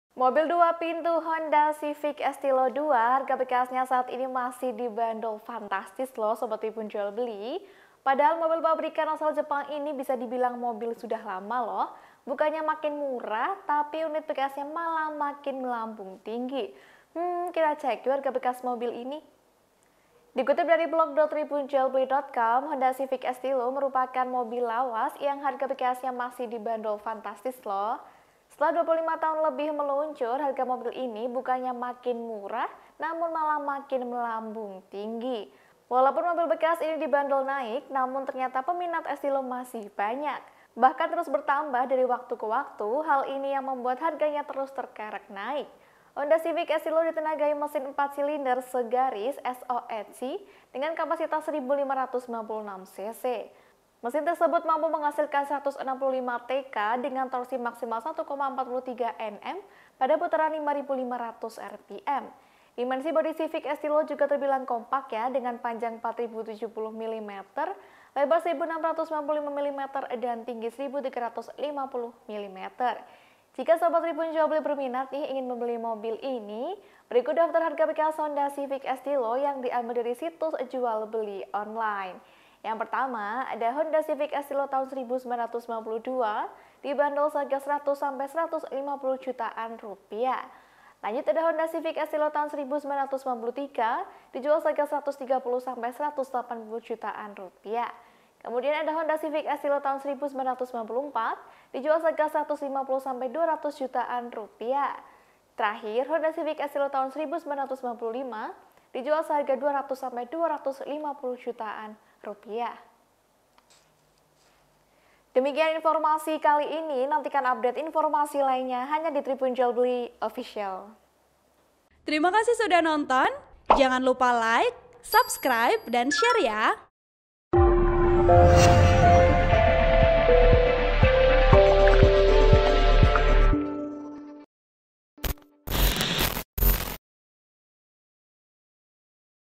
REHAT: Mobil Lawas Ini Masih Dibanderol Harga Fantastis, Cek Harga Bekas Honda Civic Estilo 2 Pintu
TRIBUN-VIDEO.COM - Mobil dua pintu Honda Civic Estilo 2 harga bekasnya saat ini masih dibanderol fantastis.

Padahal mobil pabrikan asal Jepang ini bisa dibilang mobil sudah lama.

Bukannya makin murah, tapi unit bekasnya malah makin melambung tinggi. Kita cek harga bekas mobil ini.

Dikutip dari Blog.tribunjualbeli.com, Honda Civic Estilo merupakan mobil lawas yang harga bekasnya masih dibanderol fantastis.

Setelah 25 tahun lebih meluncur, harga mobil ini bukannya makin murah, namun malah makin melambung tinggi.

Walaupun mobil bekas ini dibanderol naik, ternyata peminat Estilo masih banyak.

Bahkan terus bertambah dari waktu ke waktu, hal ini yang membuat harganya terus terkerek naik.

Honda Civic Estilo ditenagai mesin empat silinder segaris SOHC dengan kapasitas 1.596 cc.

Mesin tersebut mampu menghasilkan 165 tk, dengan torsi maksimal 1.43 Nm pada putaran 5.500 RPM.

Dimensi bodi Civic Estilo juga terbilang kompak dengan panjang 4.070 mm, lebar 1.695 mm dan tinggi 1.350 mm.

Jika sobat Tribunjualbeli berminat ingin membeli mobil ini, berikut daftar harga bekas Honda Civic Estilo yang diambil dari situs jual beli online.

Honda Civic Estilo 1992 dibanderol Rp 100 jutaan – Rp 150 jutaan

Honda Civic Estilo 1993 Rp 130 jutaan – Rp 180 jutaan.

Honda Civic Estilo 1994 Rp 150 jutaan – Rp 200 jutaan.

Honda Civic Estilo 1995 Rp 200 jutaan – Rp 250 jutaan.

(Tribun-Video.com)

Artikel ini telah tayang di Blog.tribunjualbeli.com dengan judul "Mobil Lawas Satu Ini Harganya Semakin Terkerek Naik, Cek Banderol Honda Civic Estilo 2 Pintu di 2022"